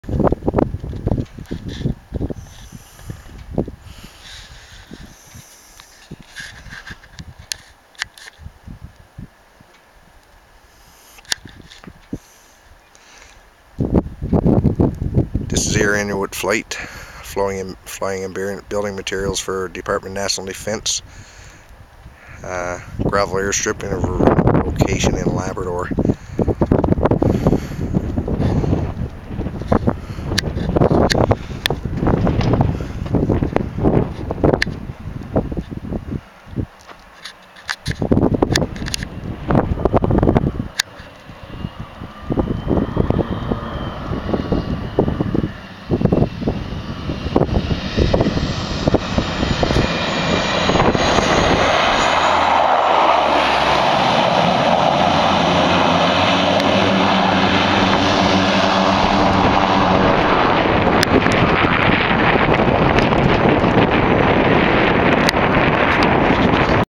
Twin otter Off strip
Out side view of my landing at a DND off strip just out side of Goose Bay.